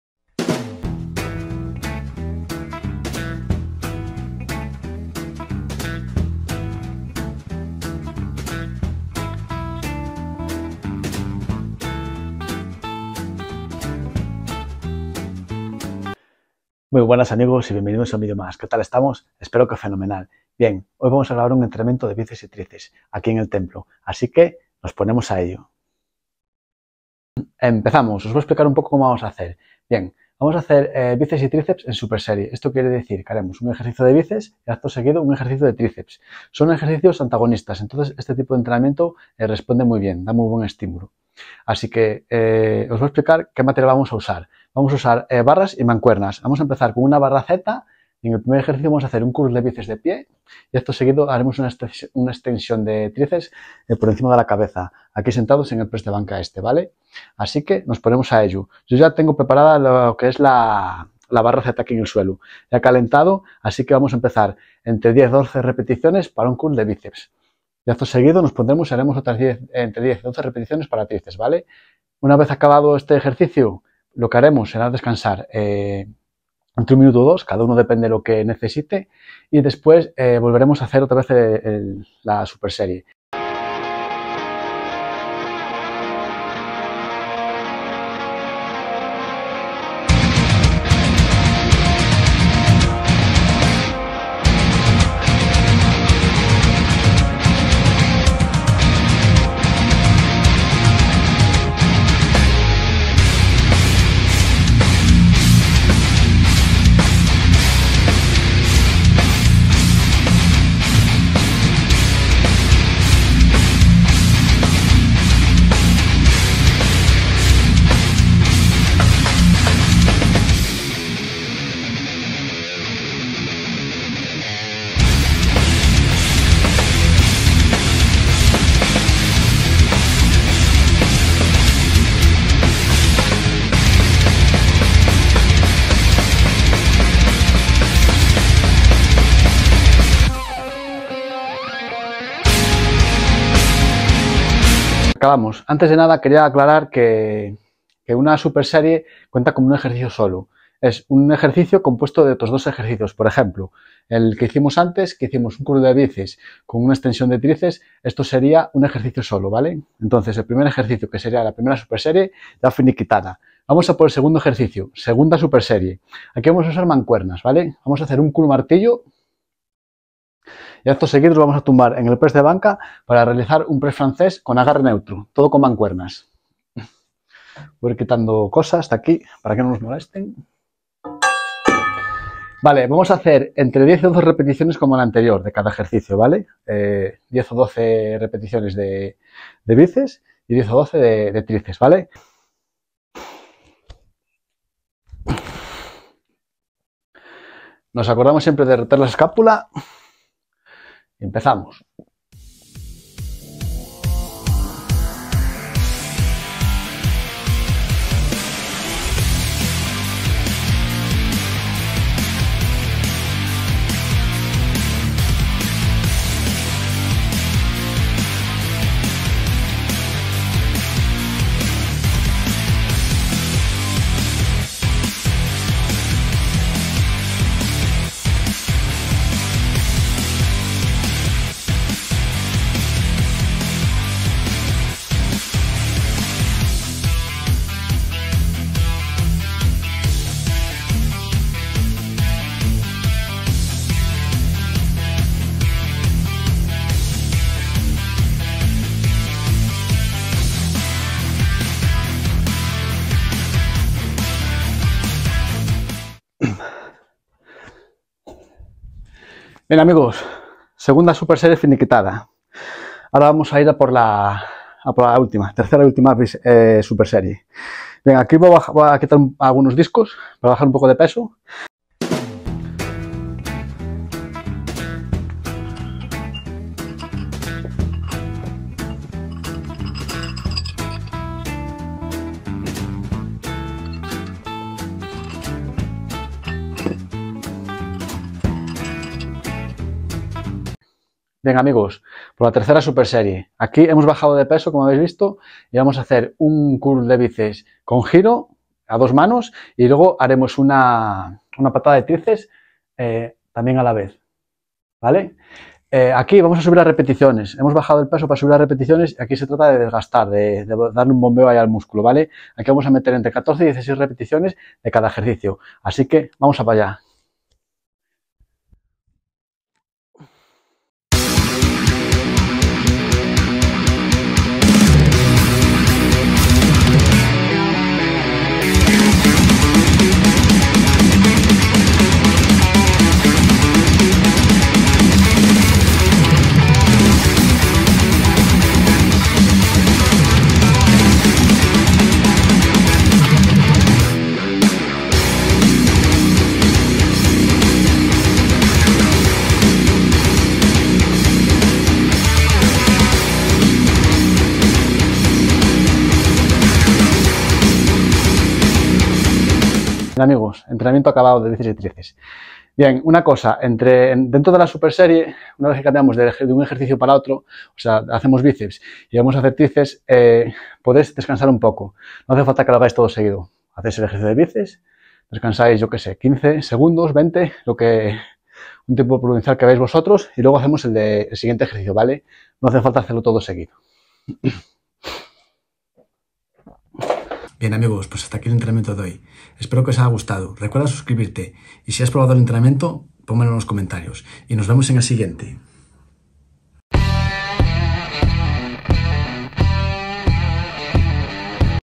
BÍCEPS y TRÍCEPS con MANCUERNAS y BARRA en CASA
Hola amigos aquí estamos con otro entrenamiento más.
En esta ocasión trabajamos bíceps y tríceps en casa con mancuernas y barra. Te va servir de mucha ayuda.

Atento a lo que se viene, estoy grabando mucho material para trabajar en casa ya que muchos me pedís este tipo de vídeos, así que vamos a darle bastante caña 💪🏻